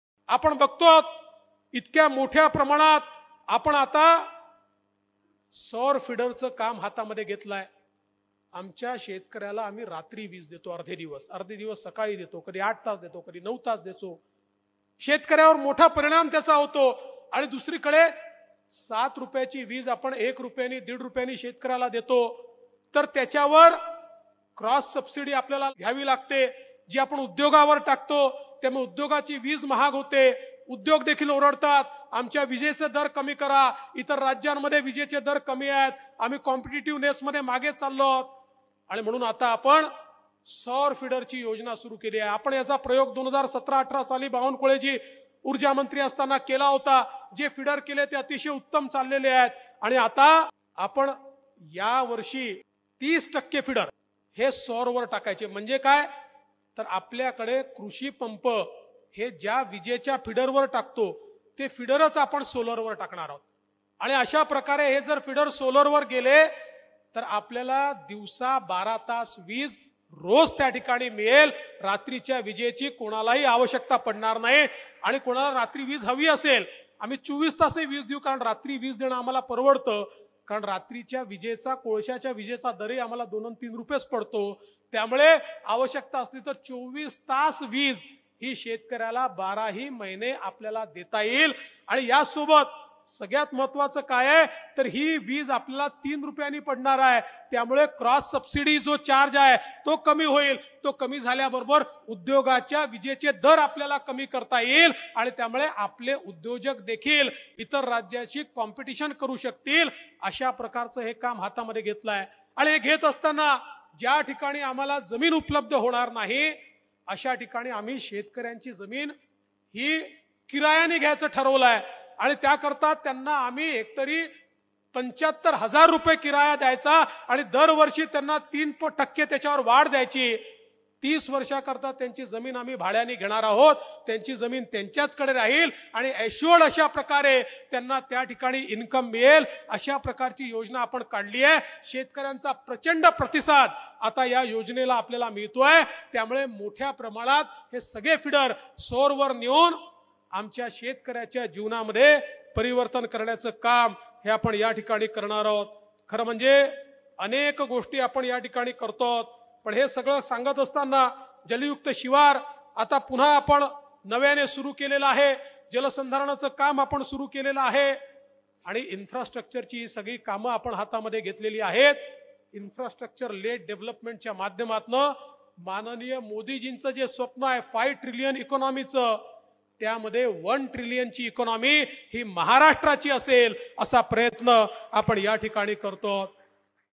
सौर फिडर योजनेमुळे 24 तास वीज, 2 लाख सौरपंपाचे उद्दिष्ट साकारणार | नाशिक | DCM Devendra Fadnavis
सौर फिडर योजनेमुळे 24 तास वीज, 2 लाख सौरपंपाचे उद्दिष्ट साकारणार | नाशिक | DCM Devendra Fadnavis | 11 फेब्रुवारी 2023